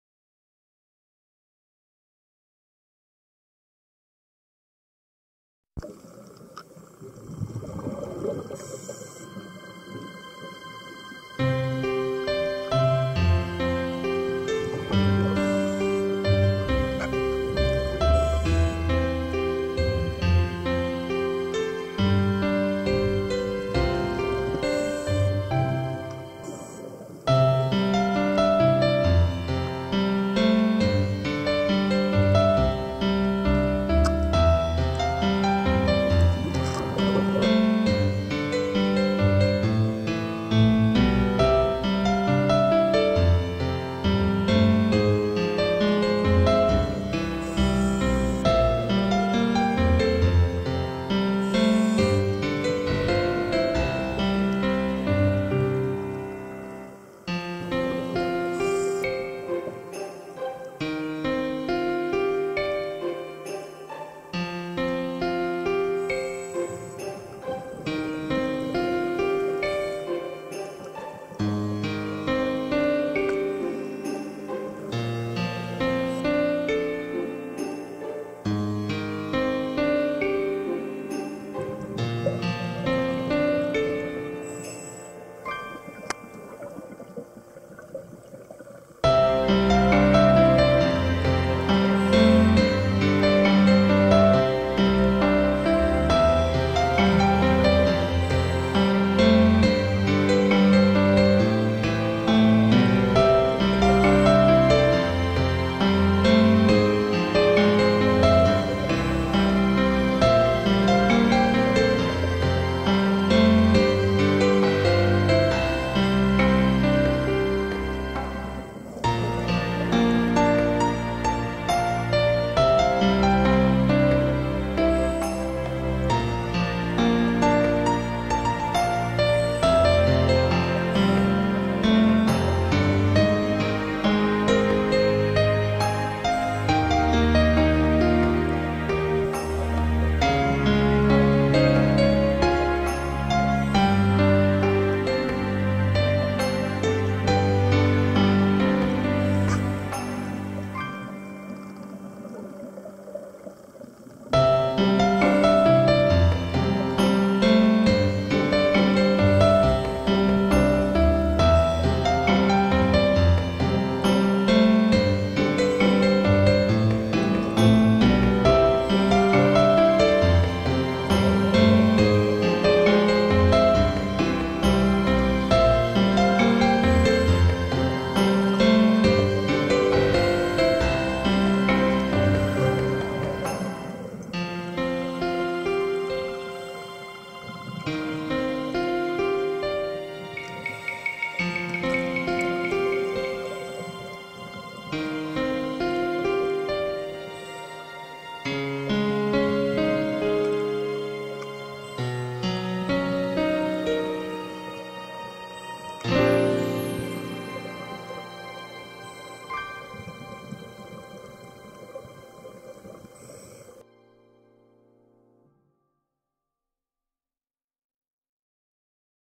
山口県 青海島 今週の海 vol.68
山口県　青海島　ダイビングショップシーアゲインです。
アオリイカの産卵やソラスズメダイの卵保護など
今週のダイビングの様子をアップいたします。